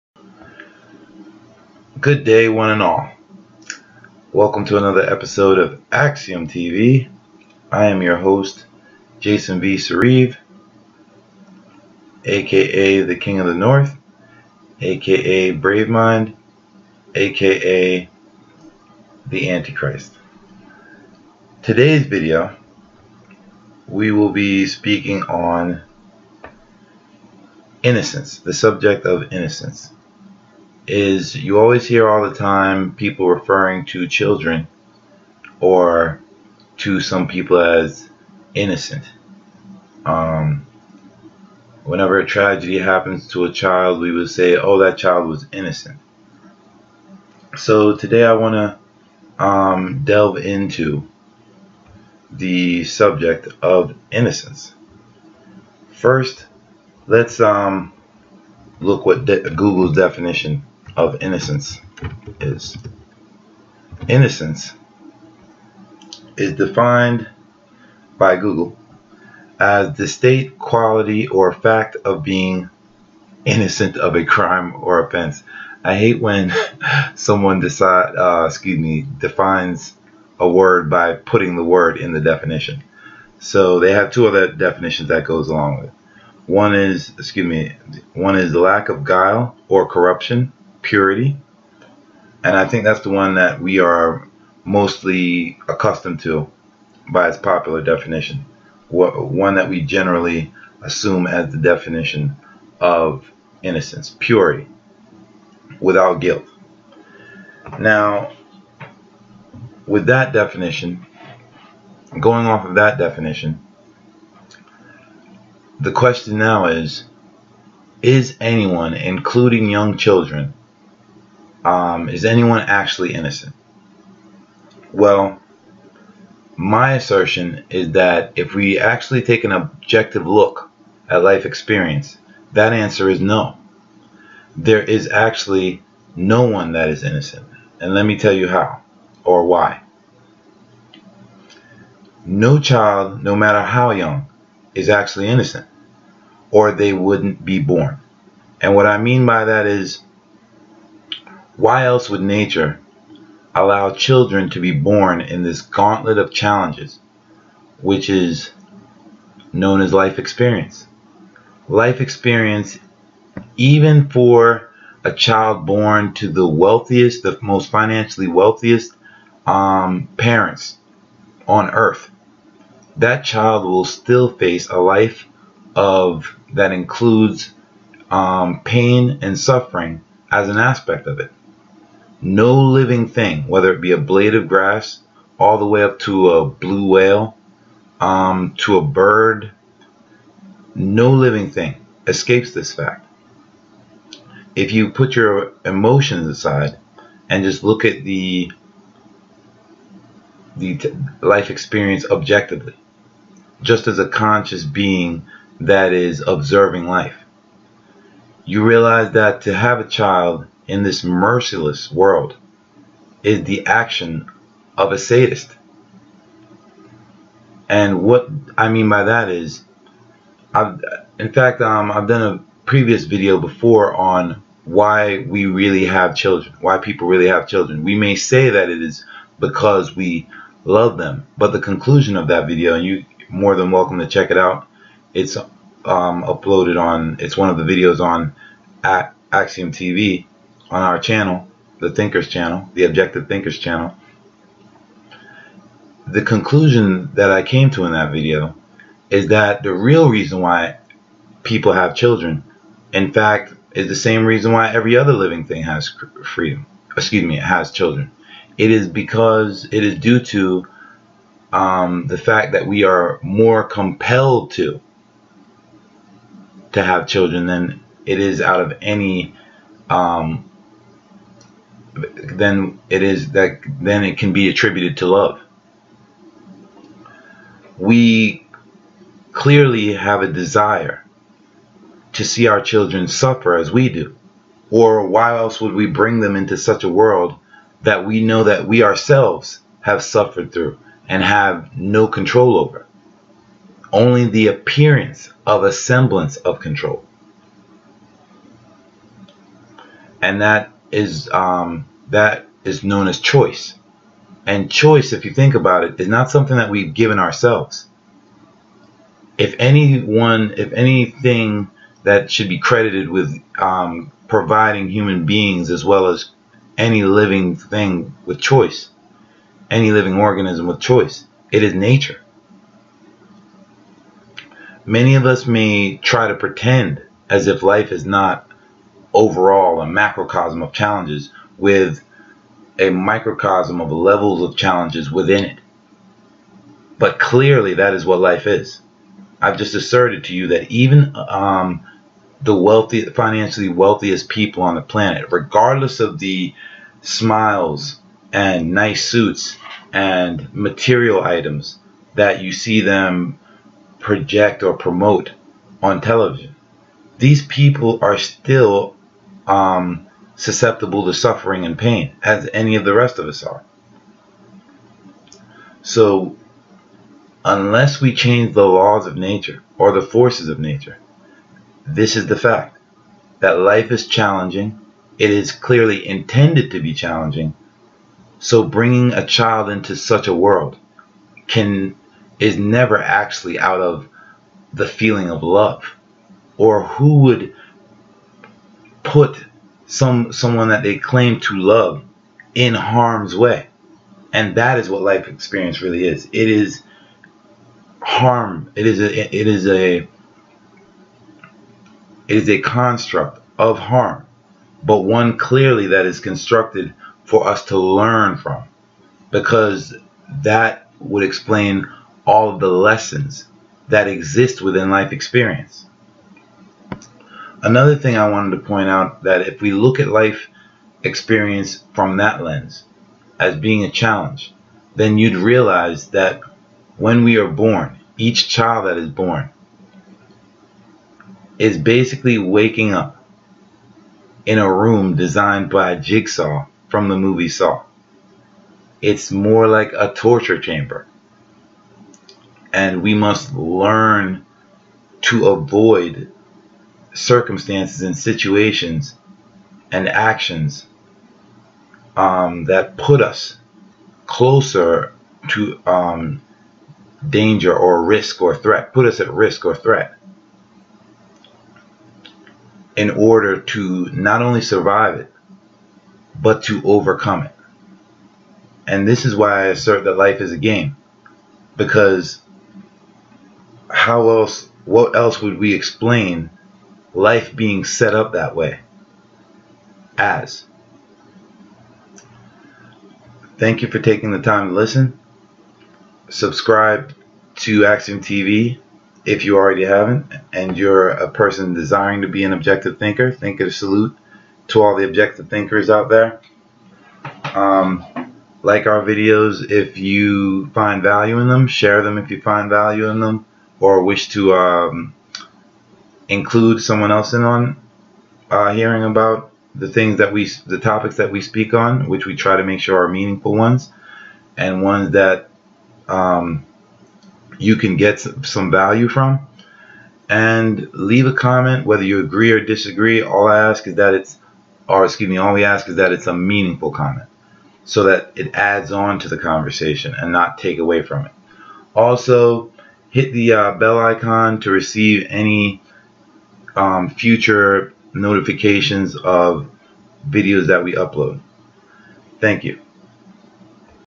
Are any children really born INNOCENT?
Exploring the question of innocence and confronting the reality of life experience.